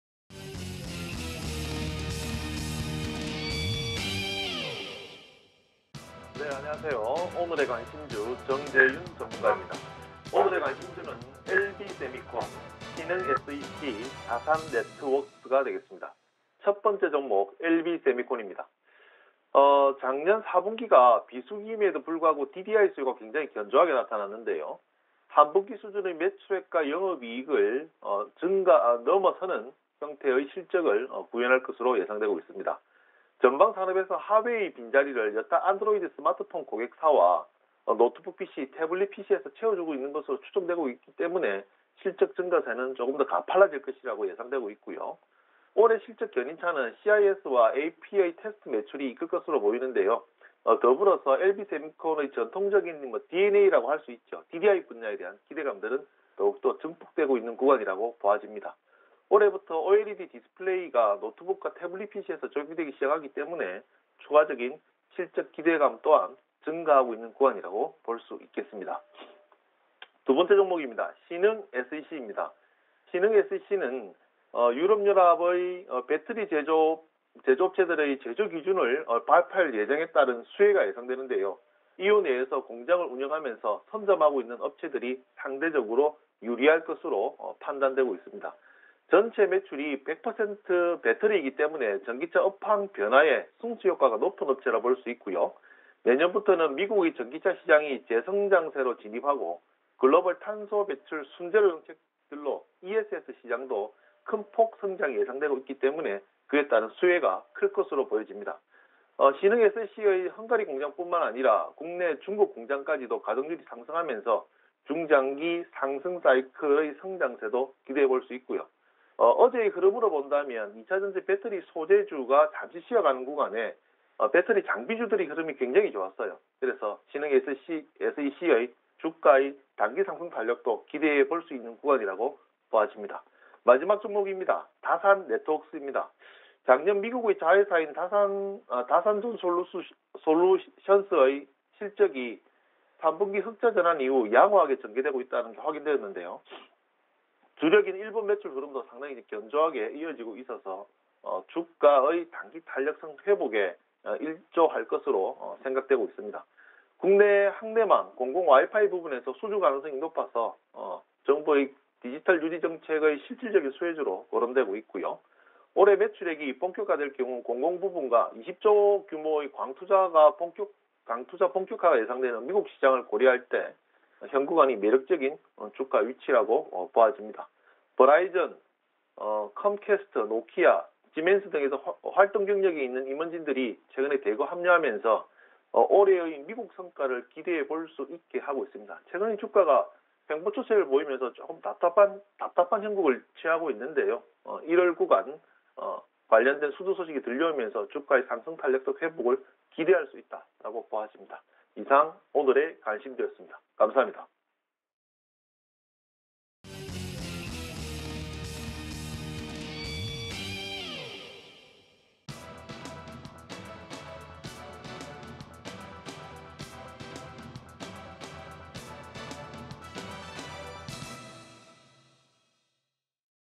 엘비세미콘, 신흥에스이씨, 다산네트웍스 (오늘의 관심주/21.01.08)
1. 엘비세미콘(061970)
작년 4분기 비수기 불구하고 DDI 수요 견조, 3분기 수준 대비 개선 예상
전방 산업에서 Huawei 빈자리를 스마트폰 고객사와 노트북 및 태블릿 PC 등 채워줌
2021년 실적 견인차는 CIS와 AP의 테스트 매출이 이끌 것
더불어 PMIC패키징 가동률 개선과 더불어 동사의 DDI 분야 기대
2021년부터 OLED 디스플레이가 노트북 및 태블릿 PC 적용 시작…실적 기대감 증가
(출처, 하나금융투자)
2. 신흥에스이씨(243840)
EU는 조만간 배터리 제조업체들의 제조기준 발표 예정
EU시장내 공장 운영 및 선점 업체들이 상대적 유리
전체 매출의 100% 배터리 때문…전기차 업황 변화 따른 승수효과 높은 업체
내년부터는 미국의 전기차 시장이 재성장세로 진입
글로벌 탄소배출 정책 인해 ESS 시장도 성장 폭 커질 것
동사의 헝가리 공장뿐 아니라 국내, 중국 공장 가동율 상승…중장기 성장싸이클 탈 것
(출처, 유진투자증권)
3. 다산네트웍스(039560)
작년 미국 자회사 'DZS' 실적 3분기 흑자전환 이후 양호하게 전개…일본 매출 흐름도 견조
국내 학내망/공공 와이파이 부문 수주 가능성 높아 디지털 뉴딜정책의 실질적인 수혜
올해 매출이 본격화될 국내 공공 부문과  20조원 규모의 광투자 본격화 예상
버라이즌, 컴캐스트, 노키아, 지멘스 등에서 활동 경력있는 임원진들 대거 합류
2021년 미국시장 성과를 기대